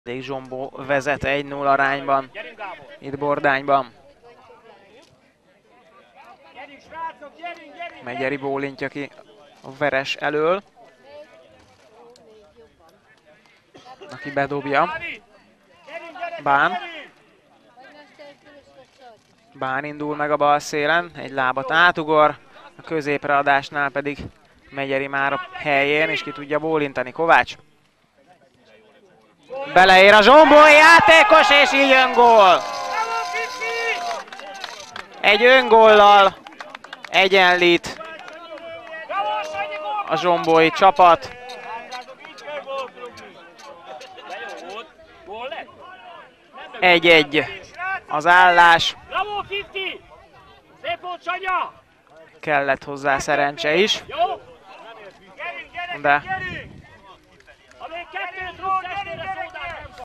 2014.10.11. Bordány-Zsombó 6-2, Bálint Vilmos öngólja (1-1)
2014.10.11., Bordány SK - Zsombó SE megyei 2. osztályú mérkőzés, Bálint Vilmos öngólja (jegyzőkönyv szerint: Kovács Sándor találata)
© Móra-net TV, 2014.
Kommentátor: dr. Németh László, operatőr: Kovács T. Norbert